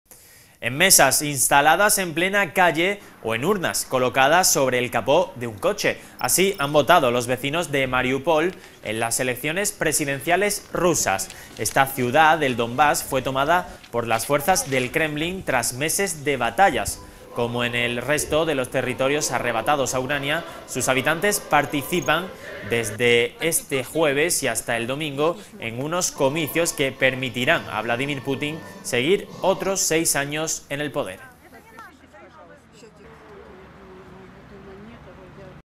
🔴Noticia - Los vecinos de Mariúpol votan en las elecciones presidenciales rusas
En mesas instaladas en plena calle o en urnas colocadas sobre el capó de un coche. Así han votado los vecinos de Mariúpol en las elecciones presidenciales rusas. Esta ciudad del Dombás fue tomada por las fuerzas del Kremlin tras meses de batallas. Como en el resto de los territorios arrebatados a Ucrania, sus habitantes participan desde hoy y hasta el domingo en unos comicios que permitirán a Vladimir Putin seguir otros seis años en el poder.
English:

At tables installed in the middle of the street or in ballot boxes placed on the hood of a car. This is how the residents of Mariupol have voted in the Russian presidential elections. This city in Donbas was taken by Kremlin forces after months of battles. As in the rest of the territories taken from Ukraine, its inhabitants are participating from today until Sunday in elections that will allow Vladimir Putin to remain in power for another six years.

Live streaming